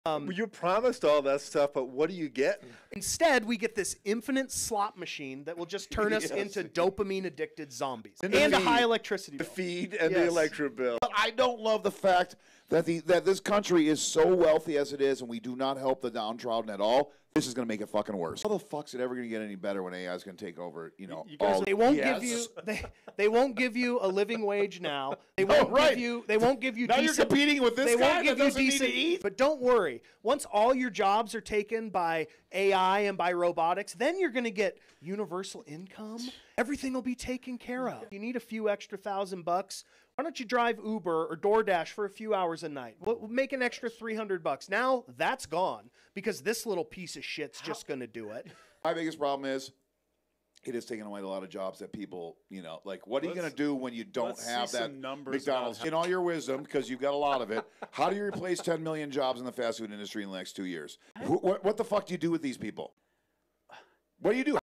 AI's Impact: Job Losses, Living Wages & Our Future
AI is predicted to displace millions of workers. What happens when machines take over fast food? Will universal basic income save us, or is it just a distraction from the real problem?